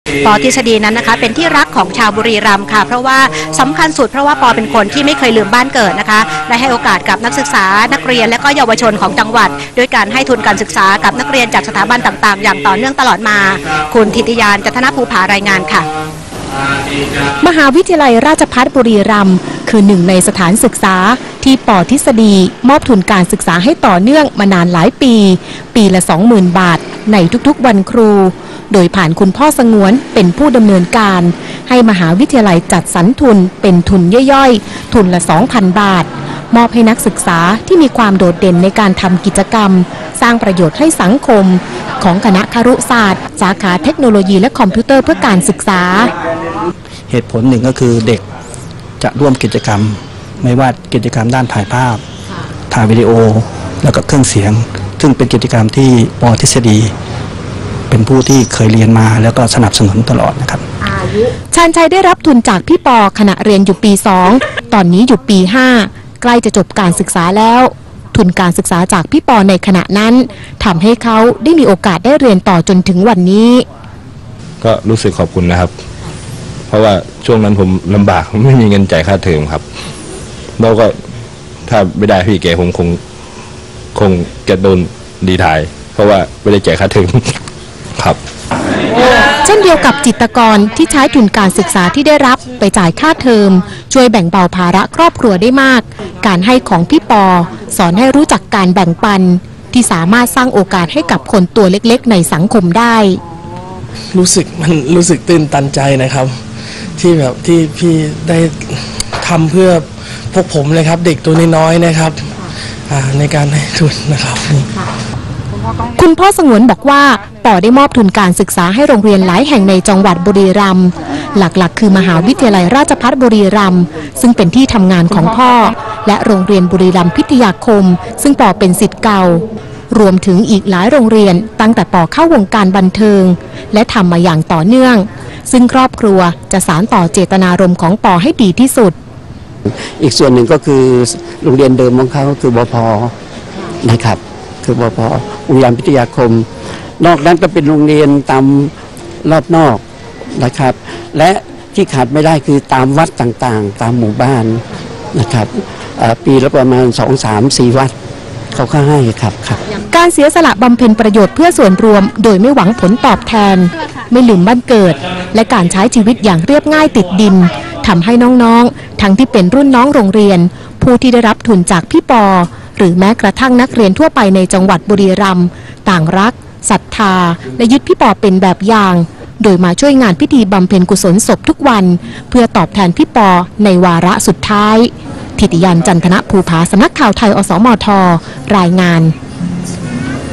ทุนพี่ปอ ต่ออนาคตเยาวชนบ้านเกิด | สำนักข่าวไทย อสมท
ปอ ทฤษฎี เป็นที่รักของชาวบุรีรัมย์ ด้วยเหตุผลสำคัญคือ ปอ เป็นคนไม่เคยลืมบ้านเกิด ให้โอกาสกับนักศึกษา นักเรียน และเยาวชนของจังหวัด ด้วยการให้ทุนการศึกษาแก่นักเรียนจากสถาบันการศึกษาต่างๆ อย่างต่อเนื่อง ติดตามจากรายงาน